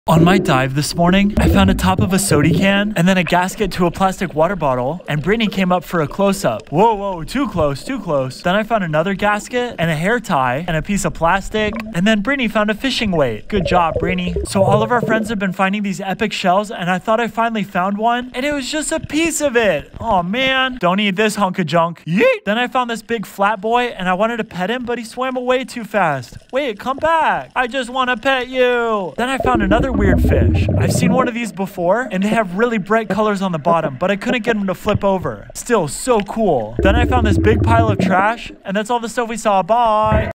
Diver picks up trash and finds cool fish
What did you think of the video? Leave a comment so we can discuss. Don't forget to SMASH dat mf like button.